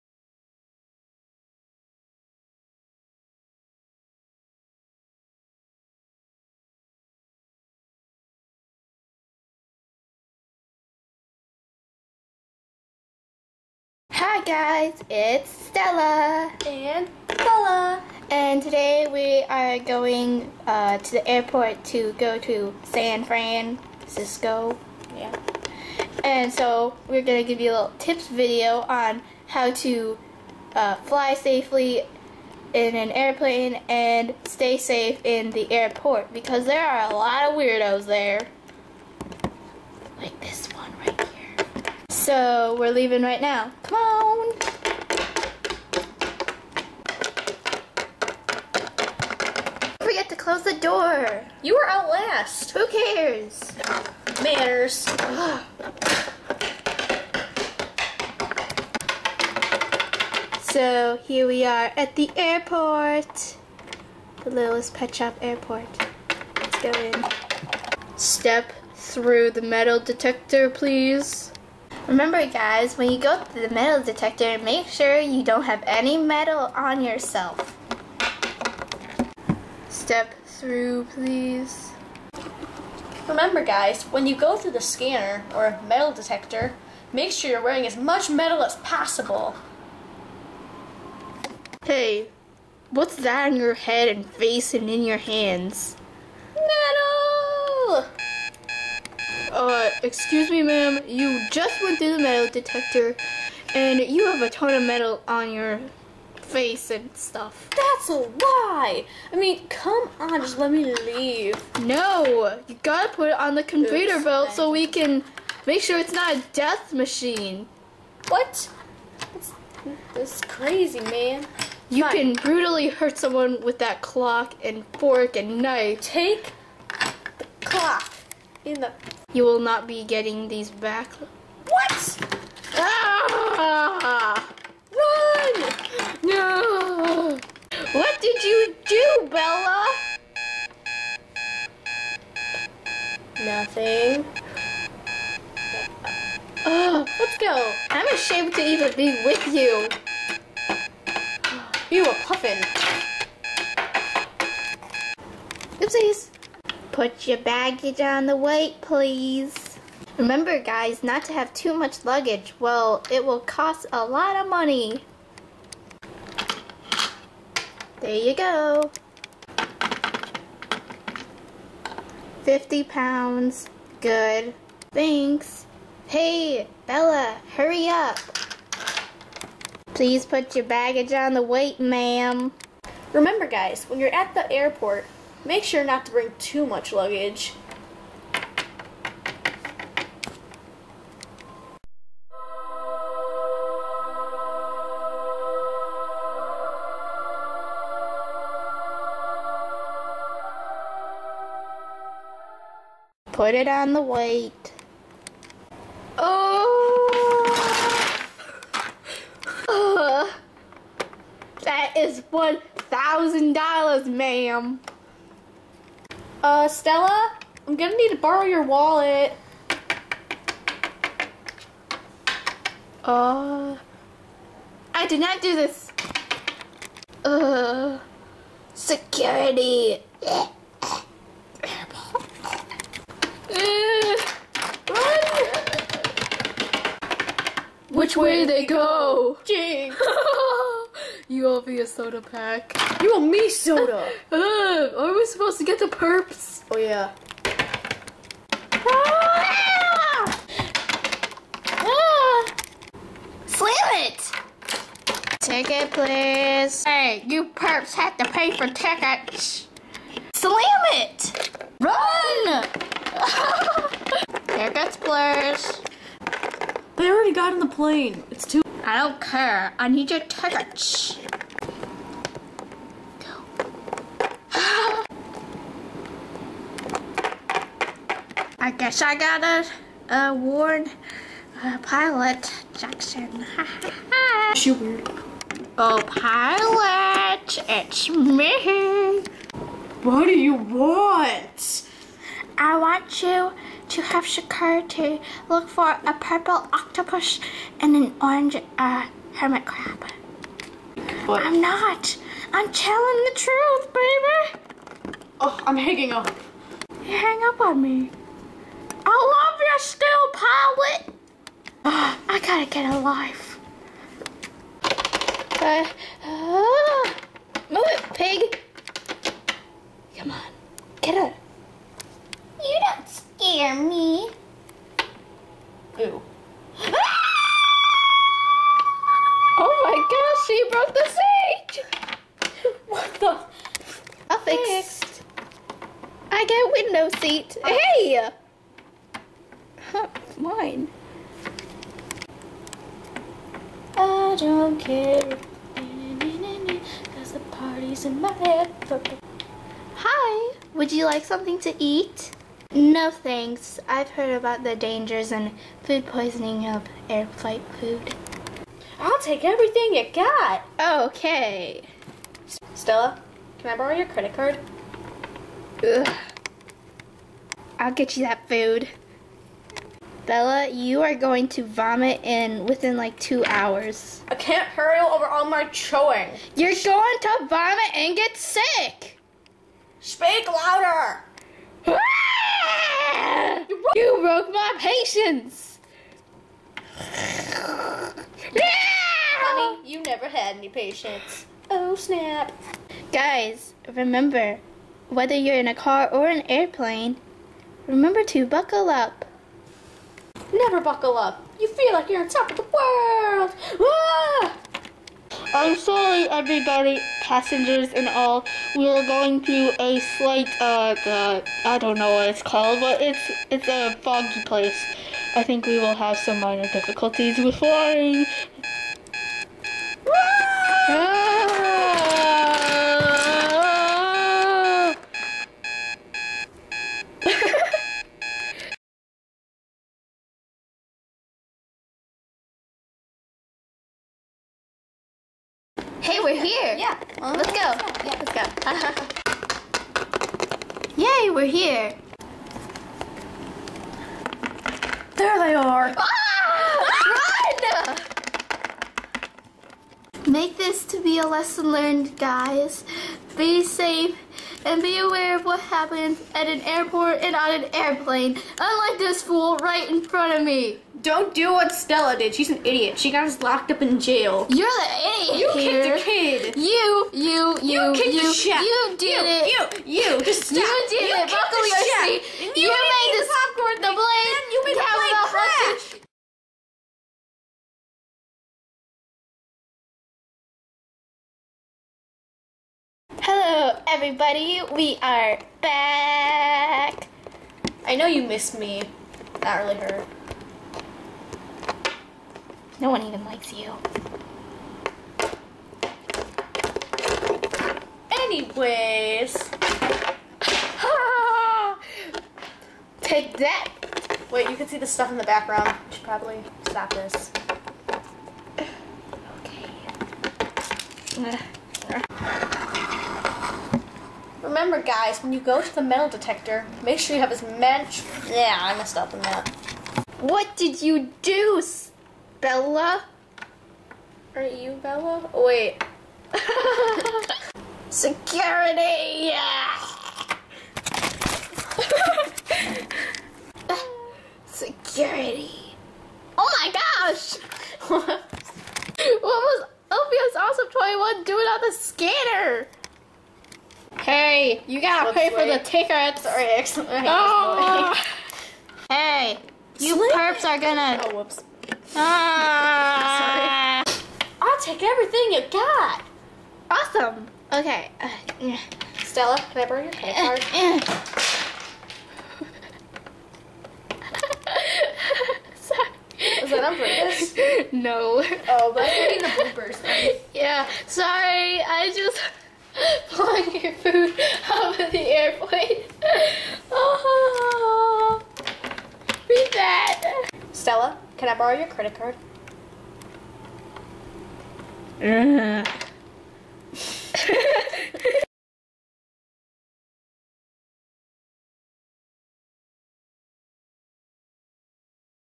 Littlest Pet Shop: Stella & Bella's Tips Airflight safety (Featuring TheDogFrogs21)
Stella & Bella are back for an all new episode! They're on their way to San Francisco (NO, I don't live there.). **Please don't attempt to reenact this video!**